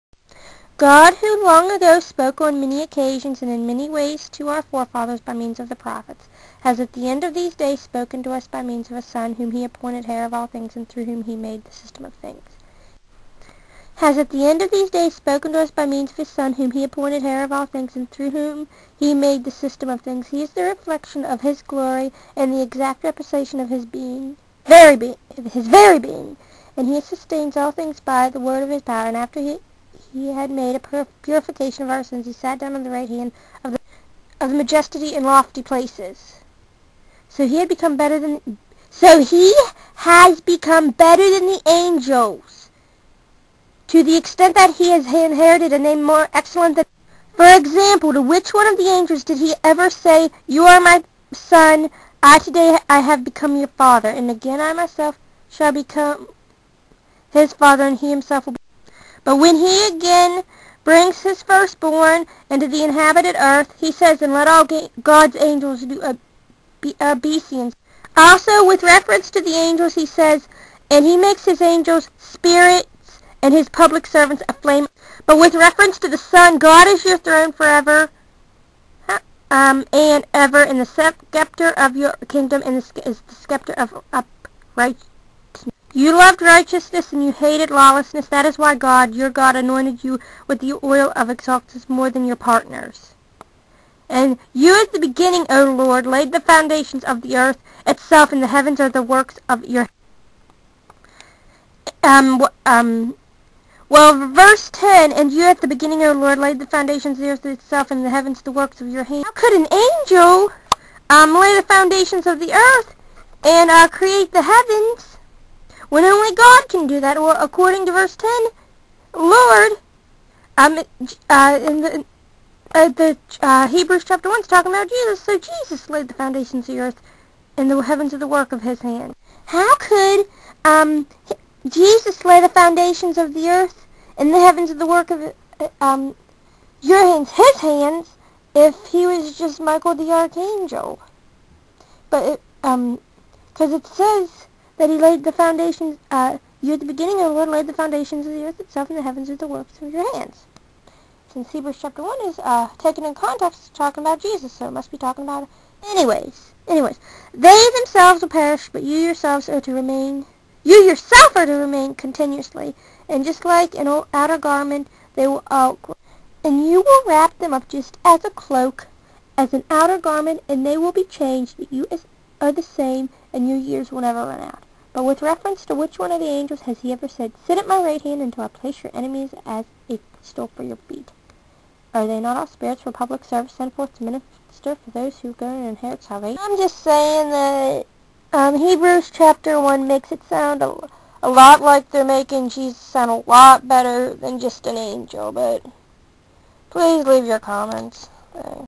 Jehovah's Witnesses and the first chapter of Hebrews
How come it sounds like, out of the NWT, that Jesus is being showed better than angels?